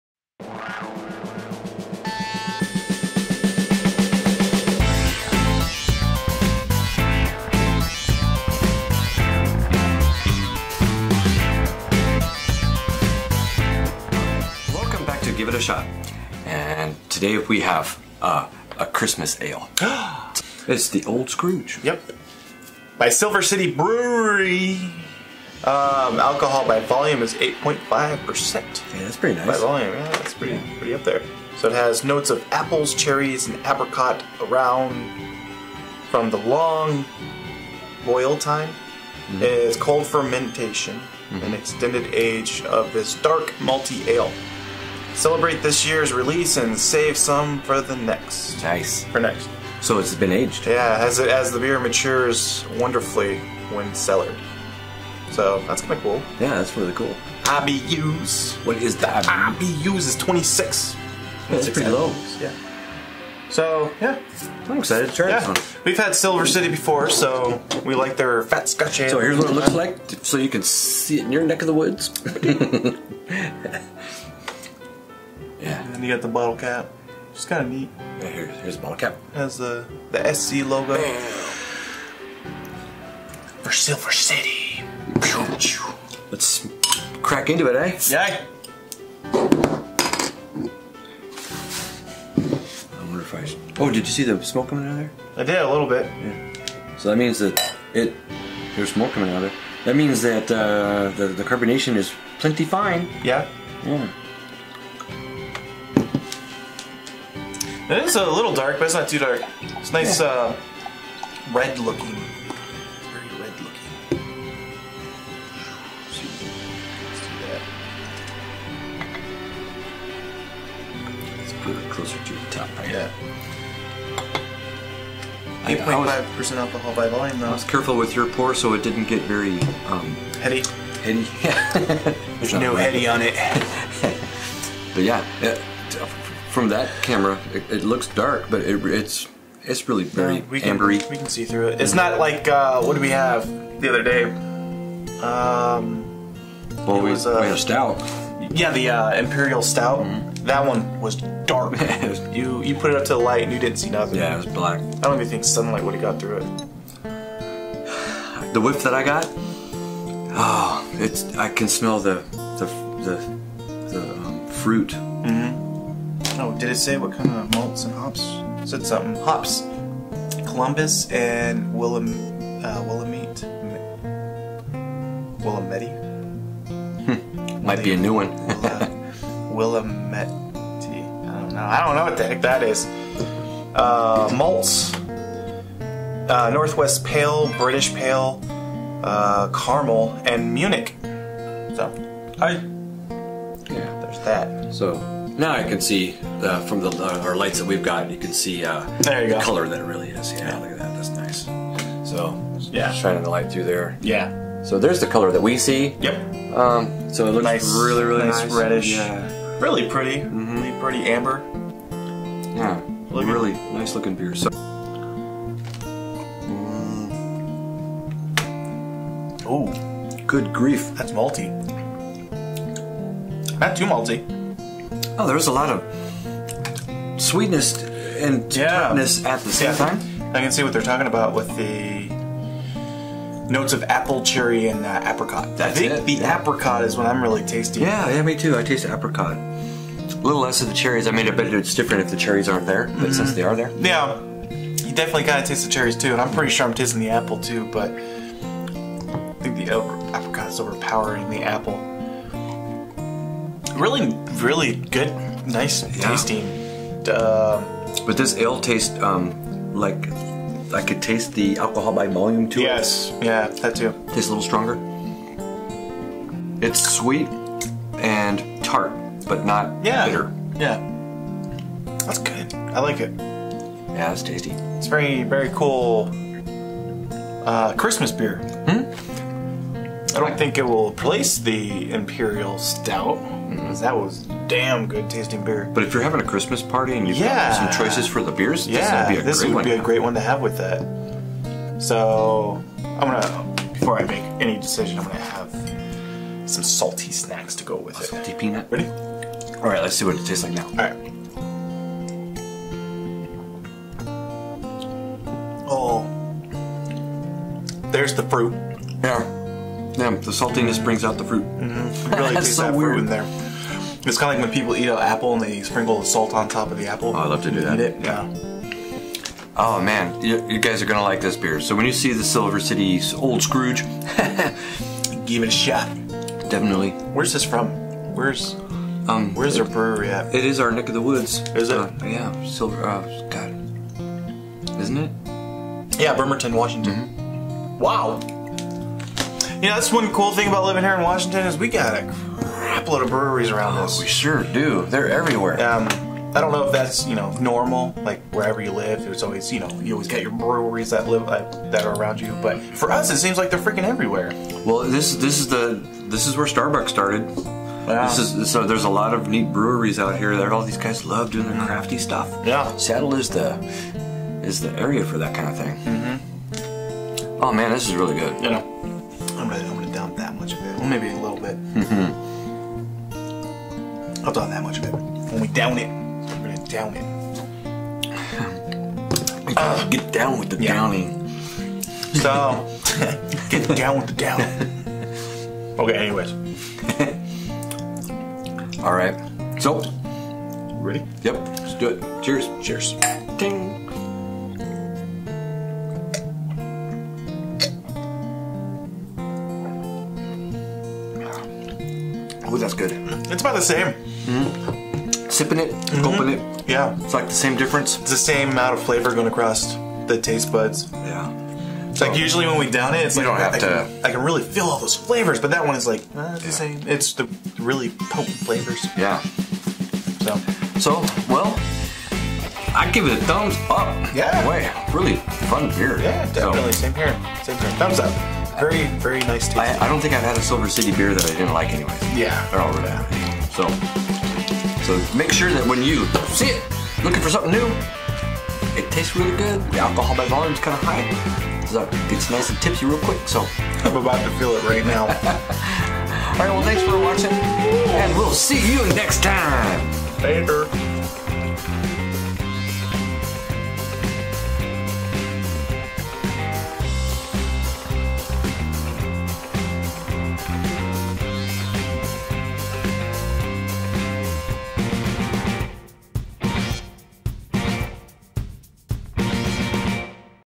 Silver City Old Scrooge on Give It A Shot
It's about that time. To day we try Silver City Old Scrooge, a Christmas ale. Lets give this one a shot. Enjoy